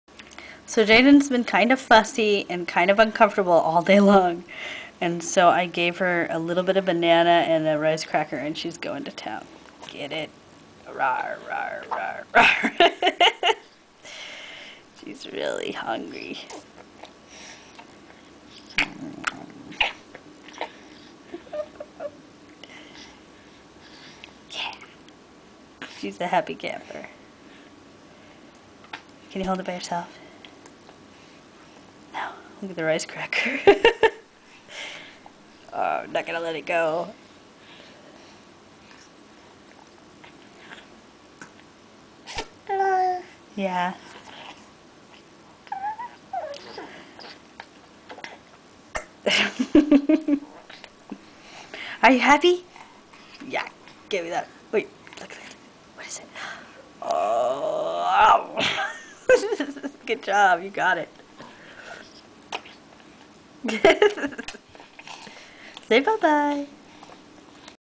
Jayden really loves banana and rice cracker!
Jayden seemed a little fussy and uncomfortable so I gave her some banana in the baby safe feeder and she went to town!!  She ate a ton of the little rice cracker and about 1/4 of a banana!  Once she was done she was happy and cooing again!